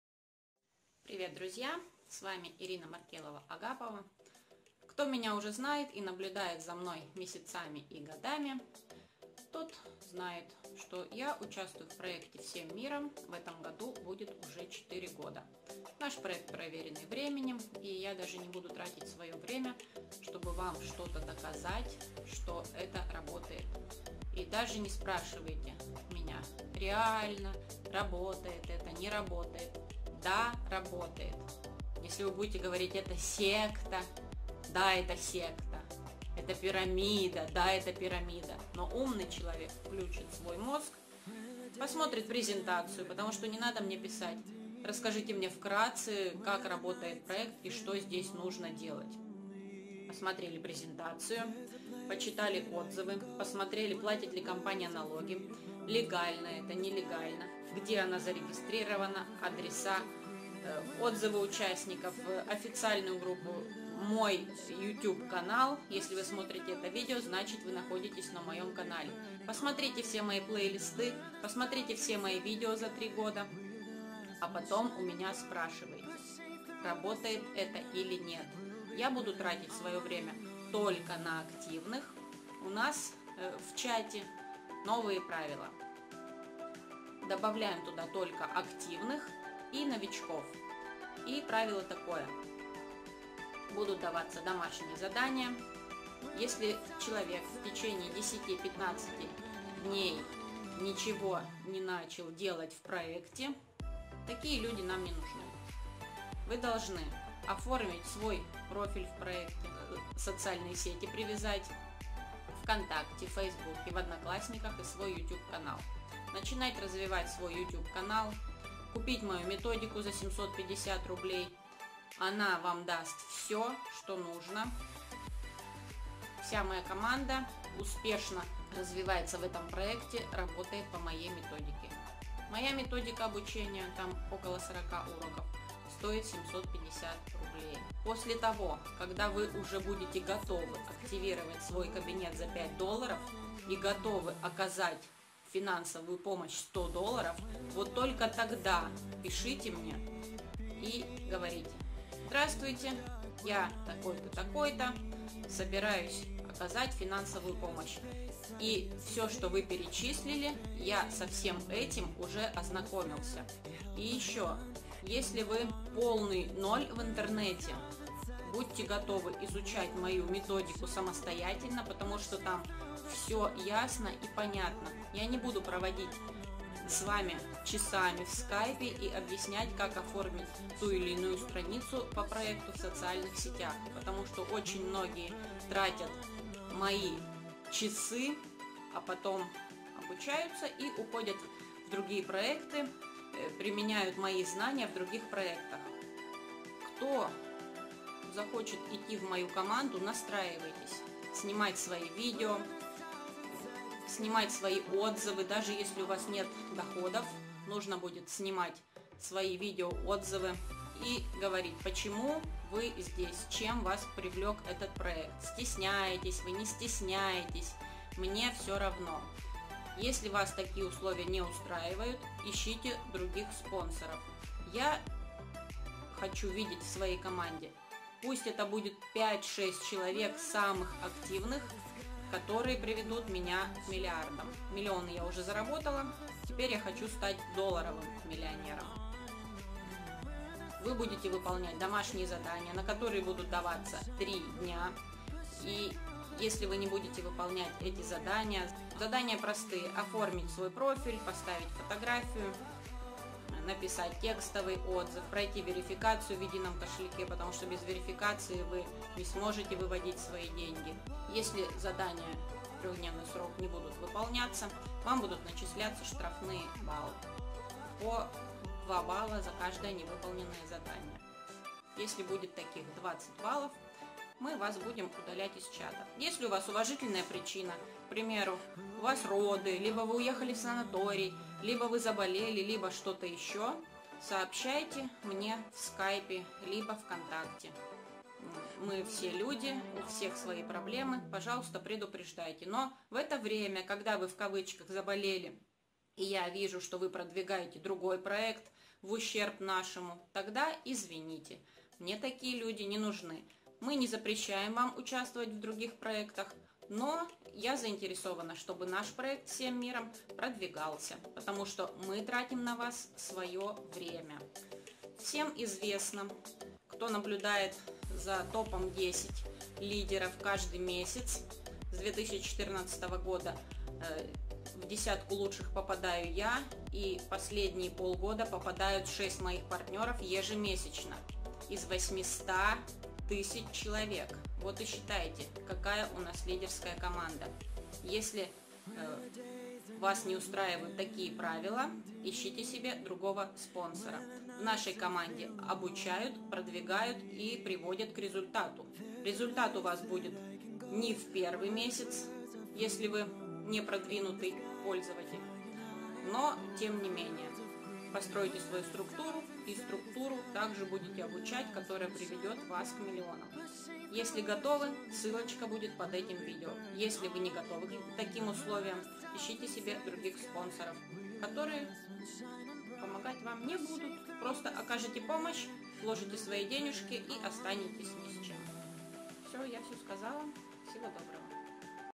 Новые правила для будущих и настоящих партнеров моей команды в проекте Whole World
Новые правила для будущих и настоящих партнеров моей команды в проекте Whole World - регистрация в мою команду